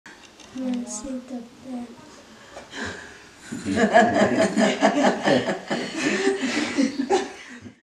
Soavina : « Merci docteur ! »
Soavina, 9 ans, soignée de la lèpre à Marana (Madagascar), a un petit message pour la Fondation Raoul Follereau 💙